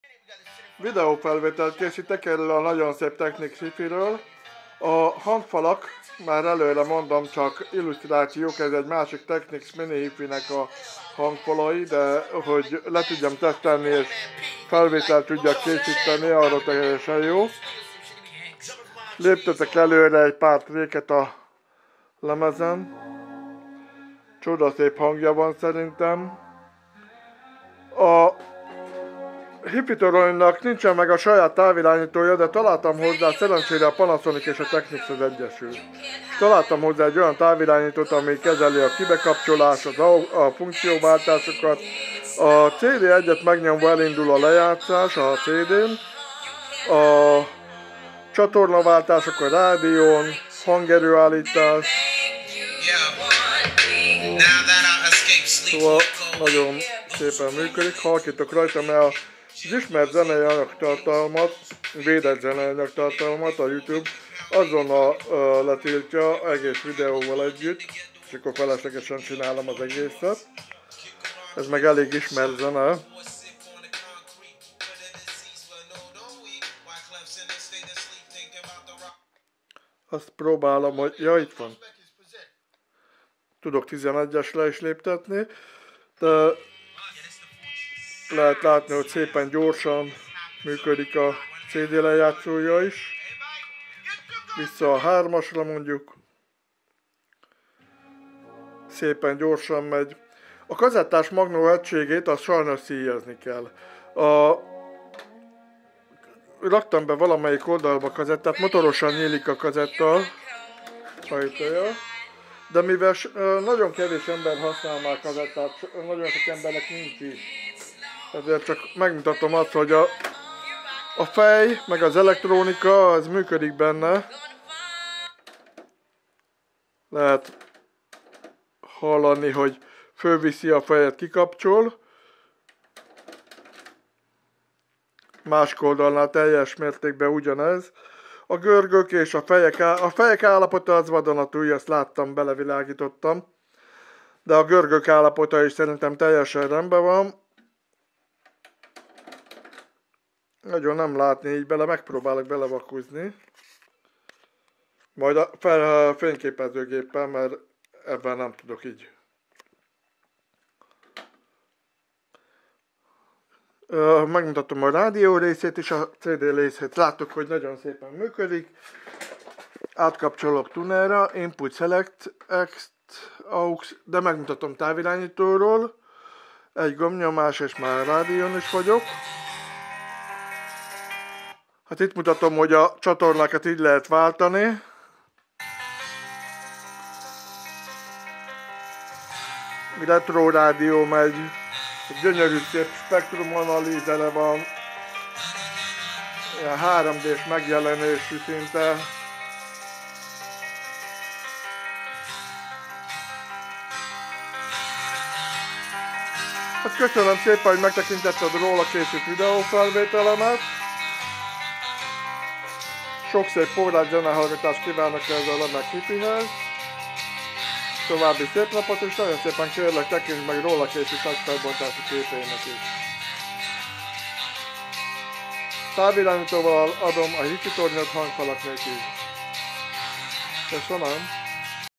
TECHNICS SA EH500 HIFITORONY MINDEN EGYSÉGE JÓ DE KAZETTÁS DECK SZÍJAZÁSRA SZORUL! EZEN KIVÜL MINDEN
TECHNICS SA EH500 HIFITORONY MINDEN EGYSÉGE JÓ DE KAZETTÁS DECK SZÍJAZÁSRA SZORÚL! EZEN KIVÜL MINDENE JÓL MŰKÖDIK MINT VIDEÓN IS ILLUSZTRÁLOM!Mivel az emberek nagy többsége már nem használ kazettát nem javíttattam. A készülék leválasztható-kivehető a toronyból! De szerintem az esztétikáját javítja így egyben ! Csoda szép hangját gyönyörű több sávos spektrum analizeréből "equalizerből"teljesen az elvárásod szerinti hangzás beállítható! A táv nem eredeti, de kezeli a CD a Rádió kazettás decket és az equalizert, de ezen csak a hang effektusokat tudjuk állítani!
TERMÉKEIM MEGVÁSÁROLHATÓAK A VATERA PIACTERÉN  bigzoleee FELHASZNÁLÓ TERMÉKEI KÖZÖTT!